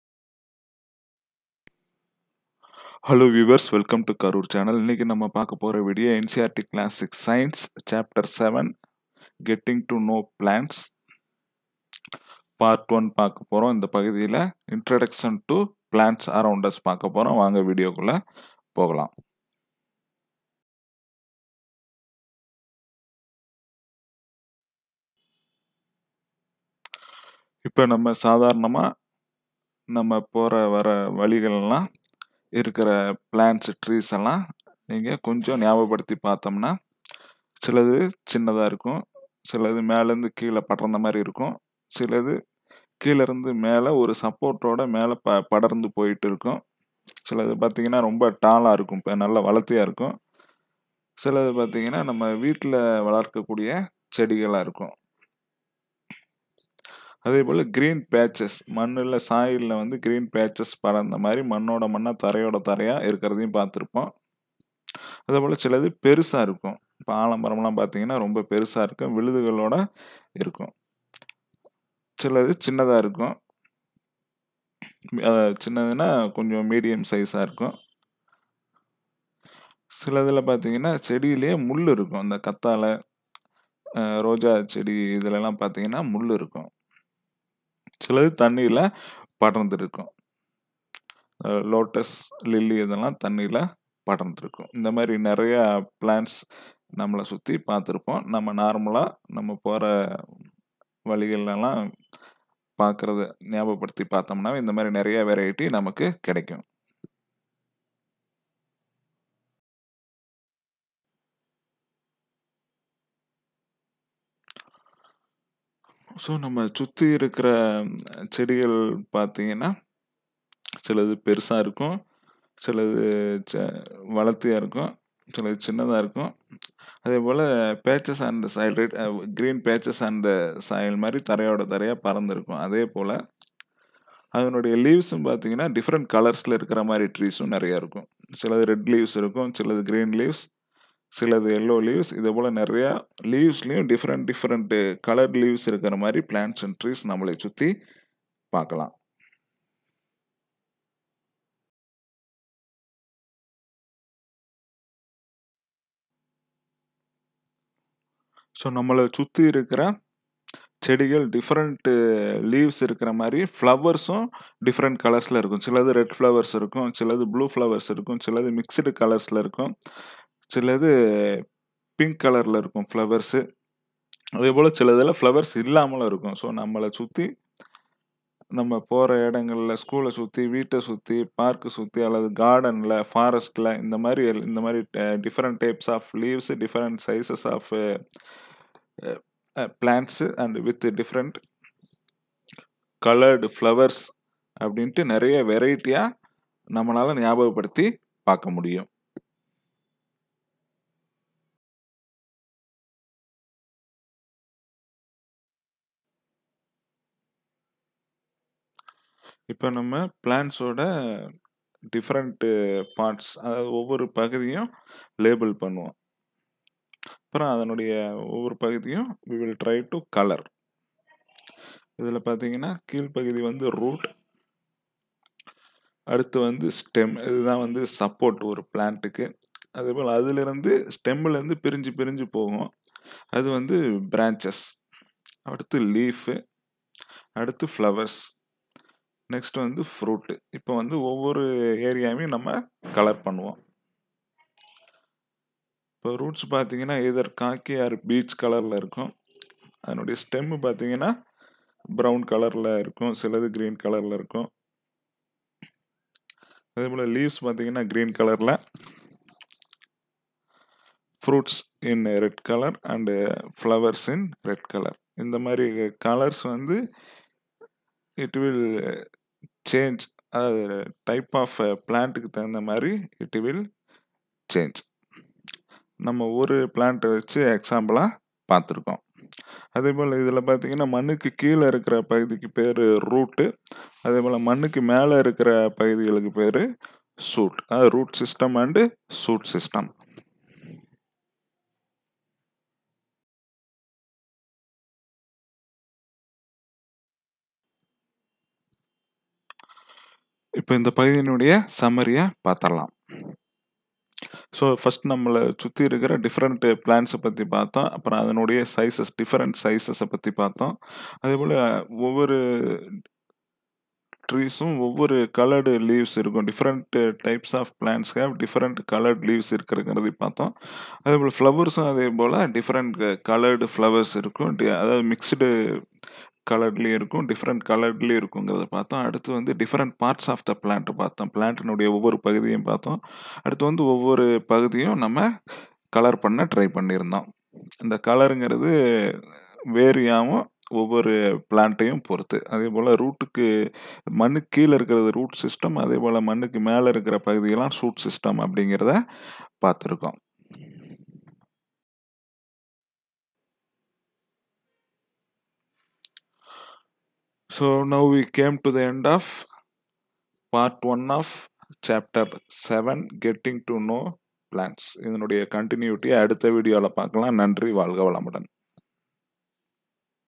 NCERT Class 6 Science in Tamil / Chapter-7 / Getting To Know Plants / Part-1 / KARUR CHANNEL
NCERT   Class-VI
Subject : Science
Chapter : 07    GETTING TO KNOW PLANTS
PART-1
                  Introduction to Plants around us

Good Luck